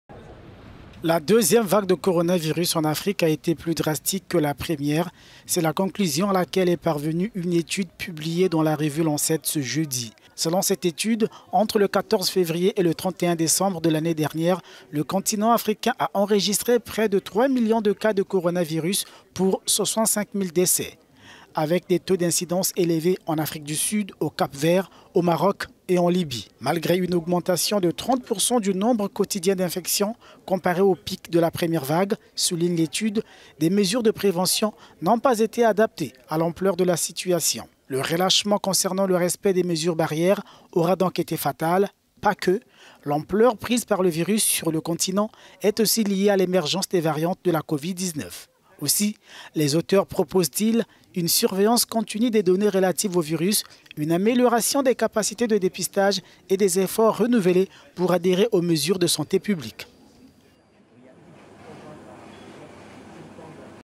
Le continent plus sévèrement touché par la deuxième vague de coronavirus
La revue The Lancet publie ce jeudi une étude sans équivoque. Plutôtpargnée par la première vague, l'Afrique a enregistré une hausse de 30% de ses infections quotidiennes ces dernières semaines.